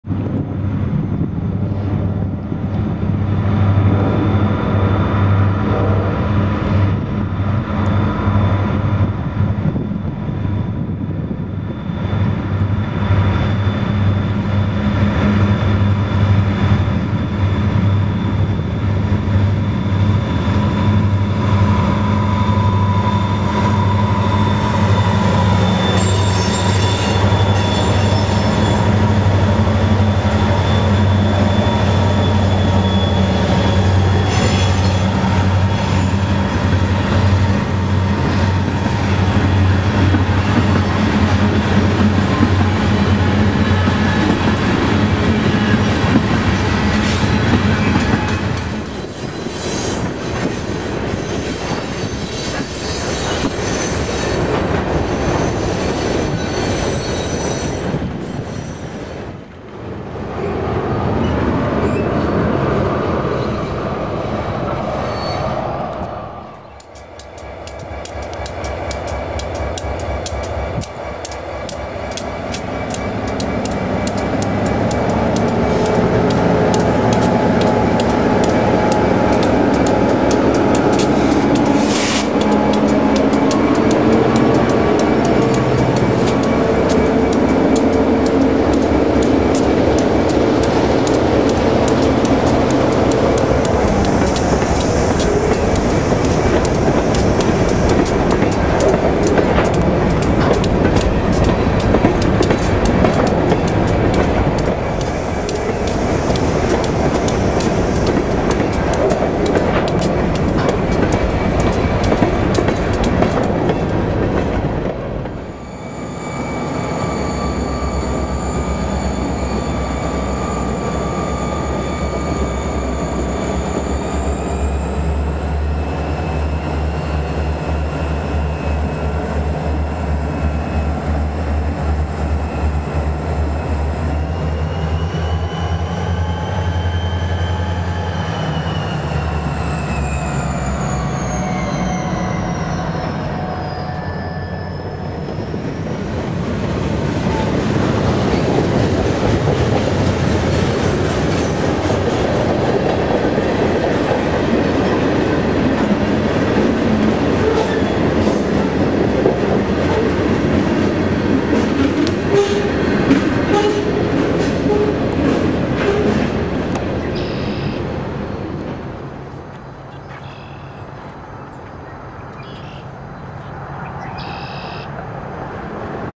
Bob Rivards Soo Line Layout - Meet at Cardigan- HO Gauge Model Trains
Lets take a step back in time to the summer of 1977 and watch train 76 meet a 937 at Cardigan Junction.  Train 76 is lead by two SD40s and a 40-2, while 937 has an impressive three F units and a SD40 dead in tow.  Once 937 cleared, train 76 got a light and got up to track speed in no time!